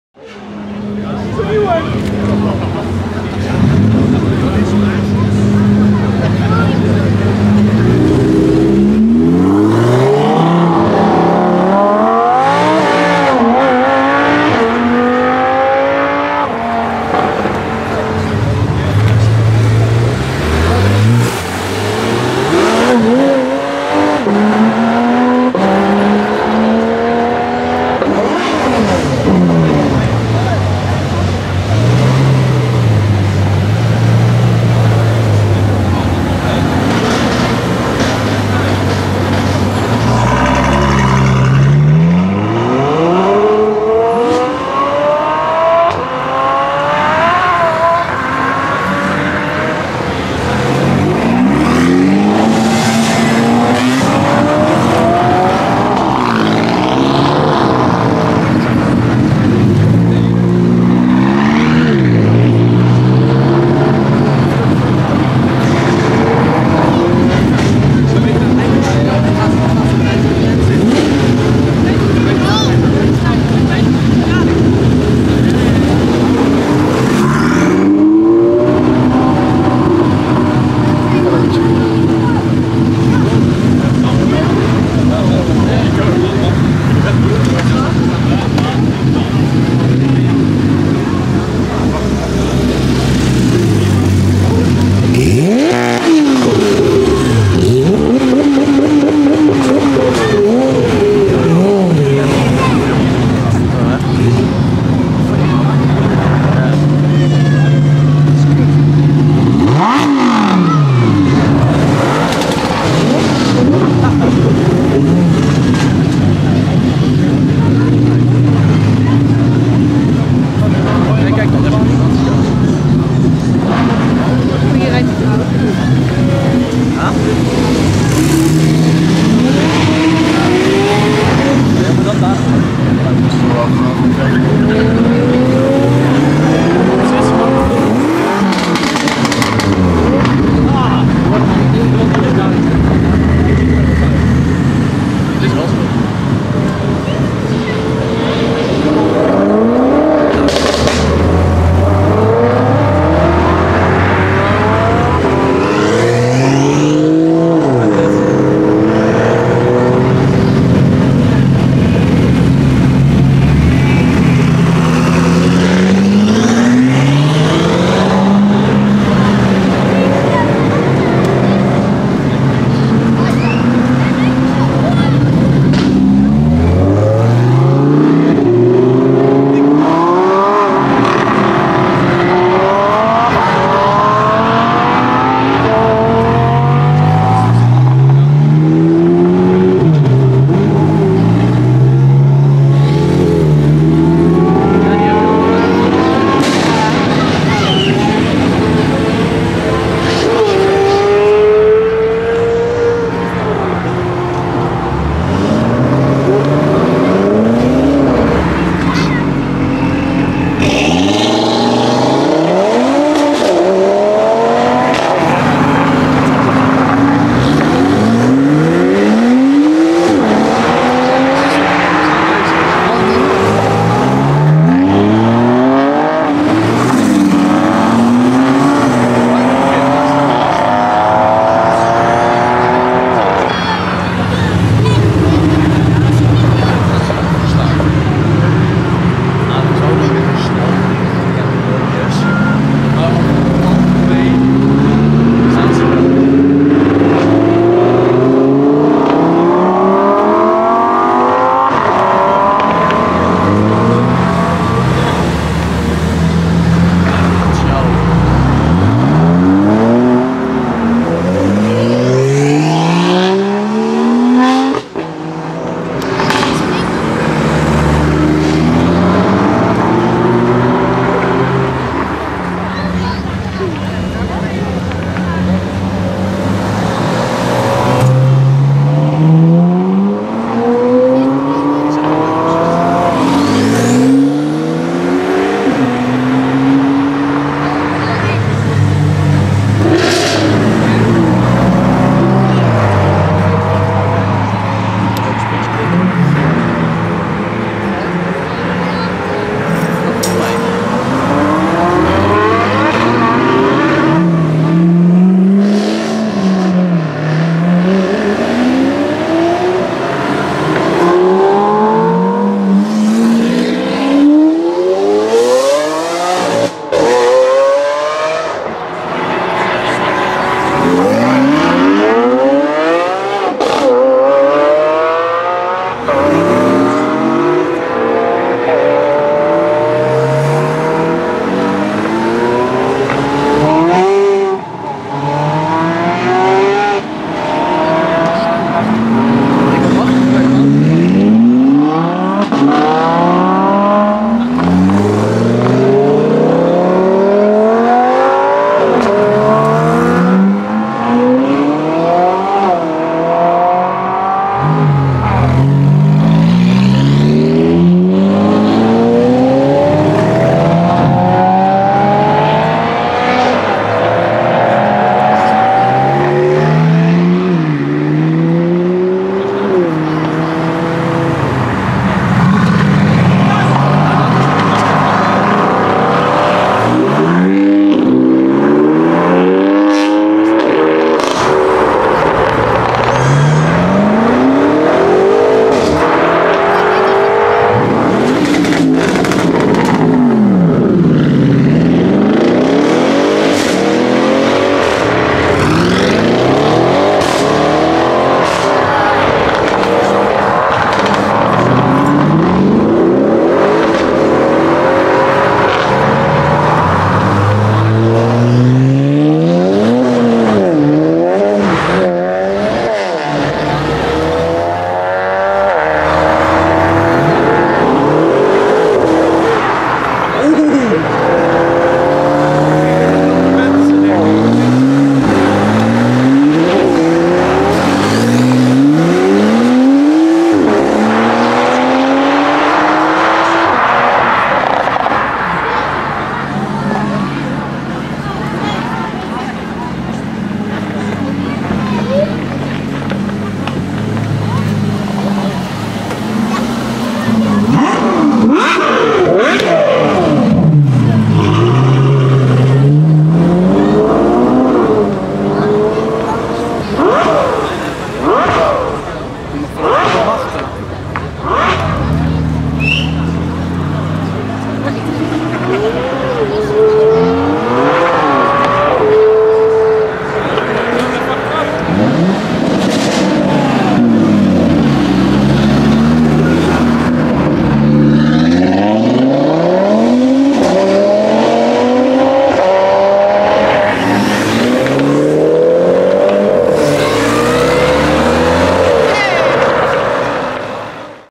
Supercars Accelerating - Capristo Aventador, SVJ, GT63, F12 N-Largo, R8 Performance & More!
During the 2019 Streetgasm 'Autumn Drive' I have filmed a lot of supercars leaving the 2nd location! You will see some crazy acceleration from many supercars! Like the Ferrari F12 N-Largo, Huracan Performante, SVJ, R8 Performance Parts, Mercedes-AMG GT63, Mosselman BMW M2 and a lot more! I hope you enjoy the video.

Ciao,
Chris /cvdzijden